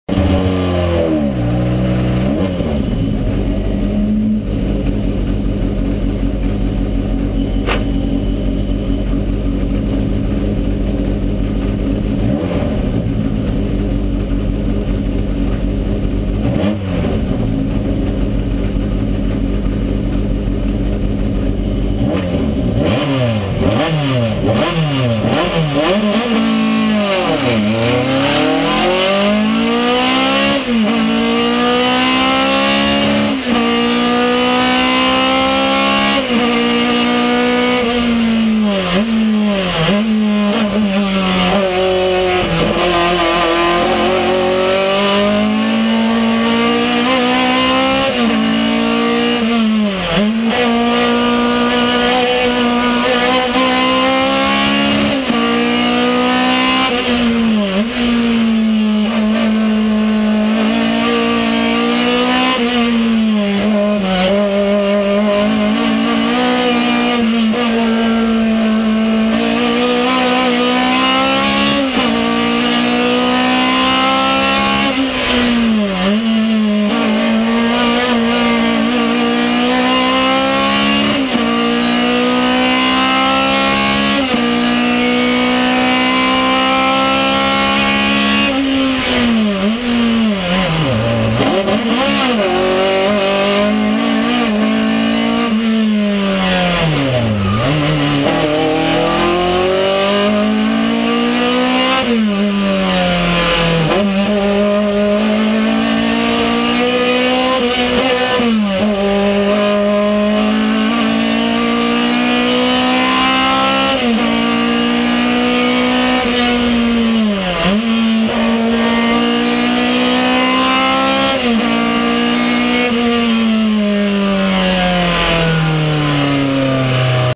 PAV Ostra luka 2011 Skoda 130LR vs. Skoda Octavia Kit Car
Skoda 130 LR E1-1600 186 ps ,Miloš Koleno sen.
 Skoda Octavia Kit Car  E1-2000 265 ps ,Miloš Koleno jun.